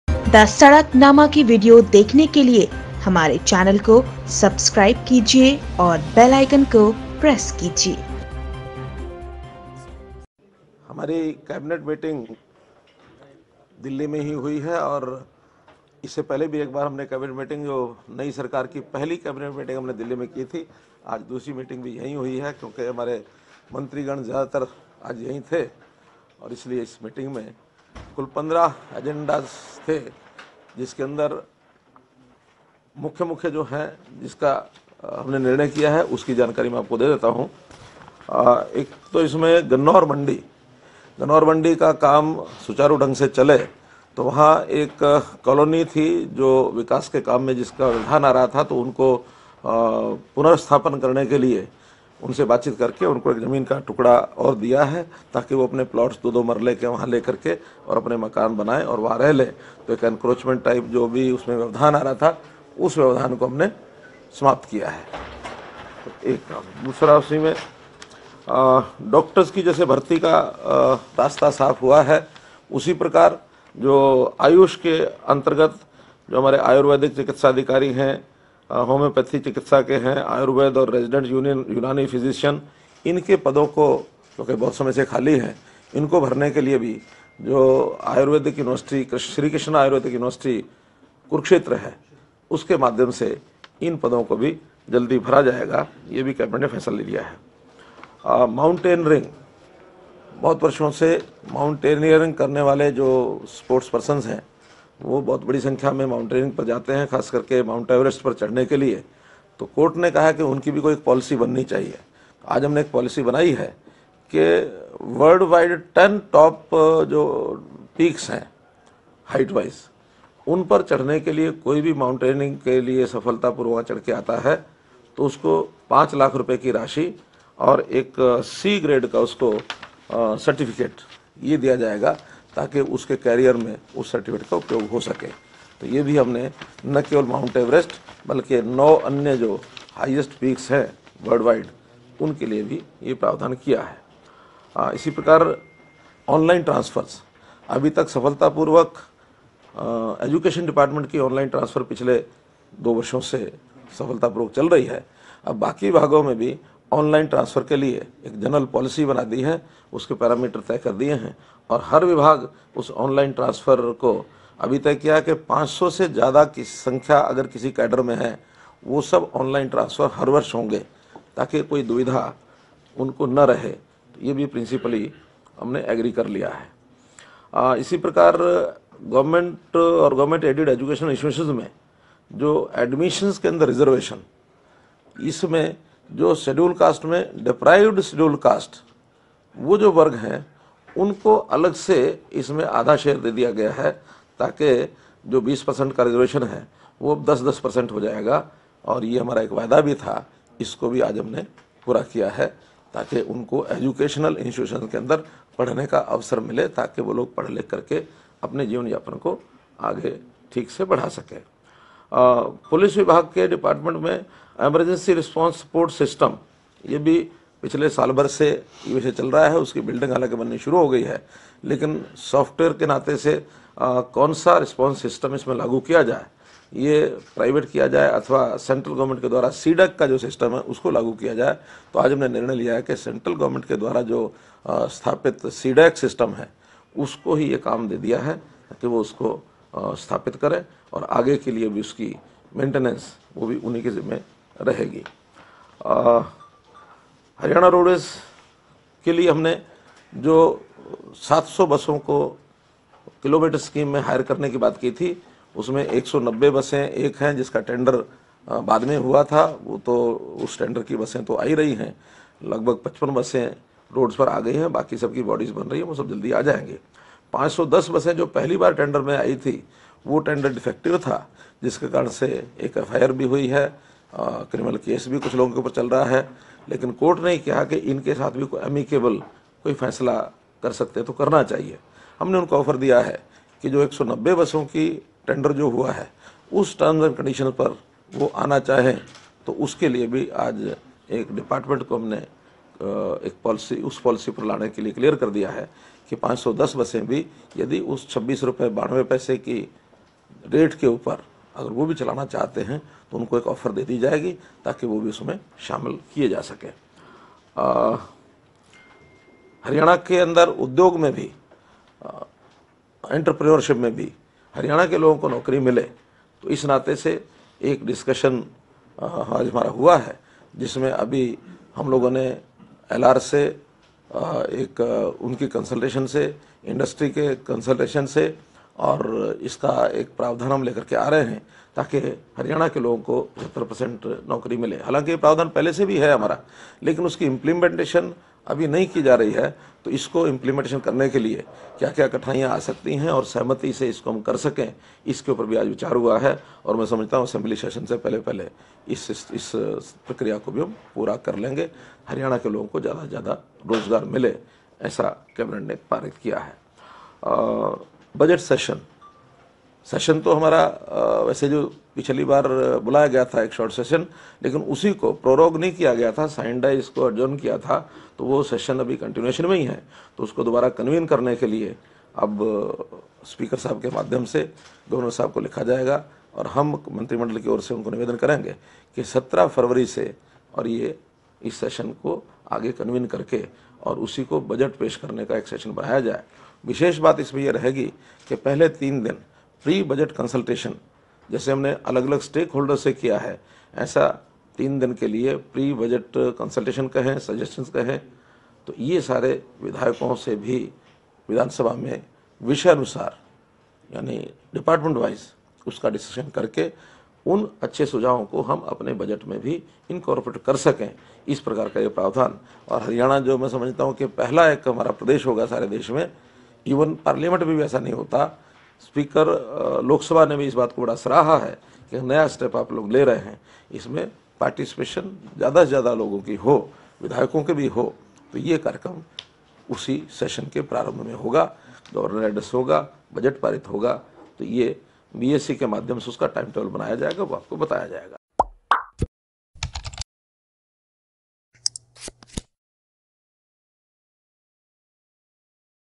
हरियाणा कैबिनेट में हुए बड़े फैसले, कई योजनाओं को हरी झंडी, अब ऑनलाइन ही होंगे ट्रांसफर!
हरियाणा कैबिनेट की मीटिंग दिल्ली में हुई, हरियाणा भवन में हुई इस मीटिंग लिए गए बड़े फैसले, कई परियोजनाओं को मिली हरी झंडी, अब प्रदेश में ऑनलाइन ही होंगे ट्रांसफर, हरियाणा के मुख्यमंत्री मनोहर लाल ने दी जानकारी, बजट सत्र फरवरी में होगा आयोजित, मार्च के पहले सप्ताह में आएगा बजट, देखिये ये रिपोर्ट प्रमोद कुमार के साथ वीडियो जर्नलिस्ट चरण सिंह।